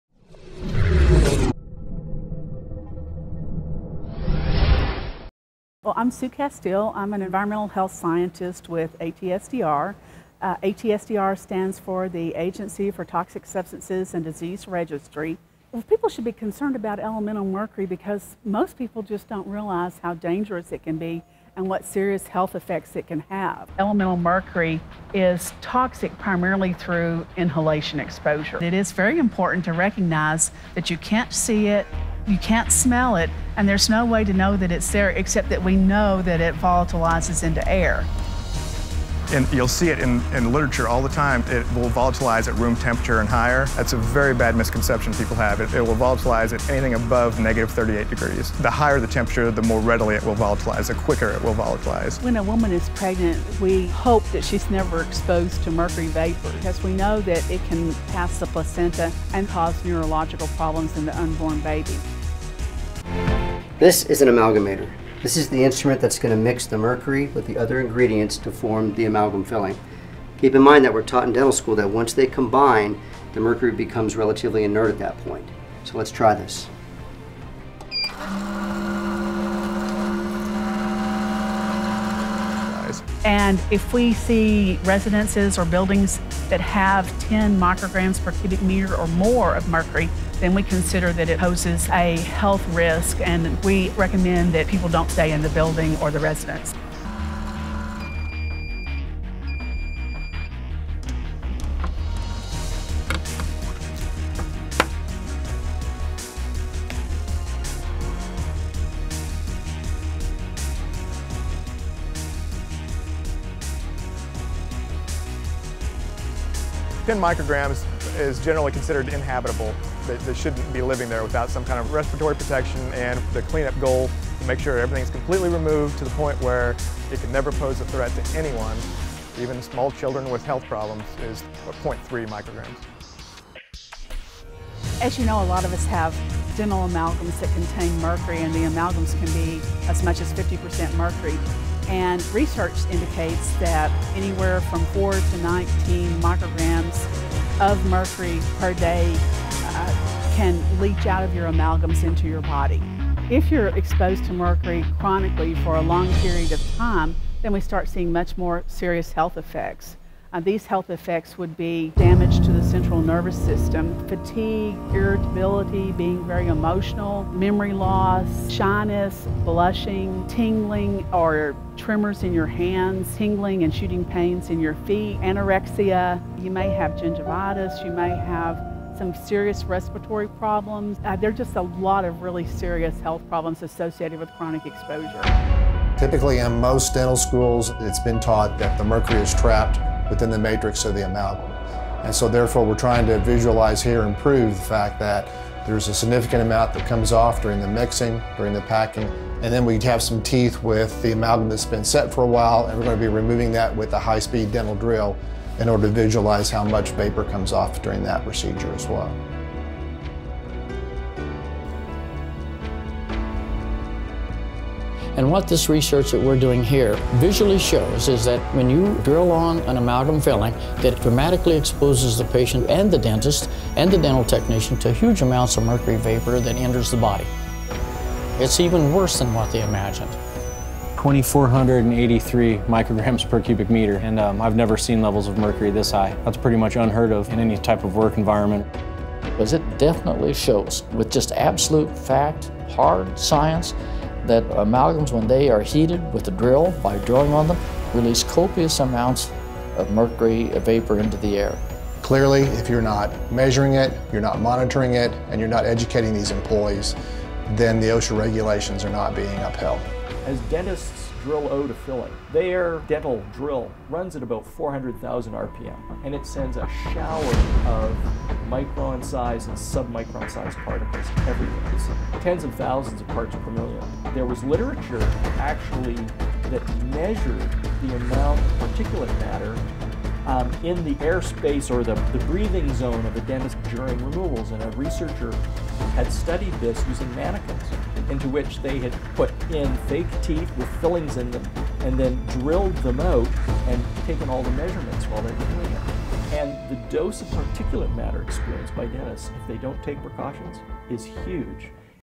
Overview of exposure and harm from mercury released from "silver" dental amalgam fillings
Overview of exposure to mercury from amalgam fillings and the harm that can come from that exposure.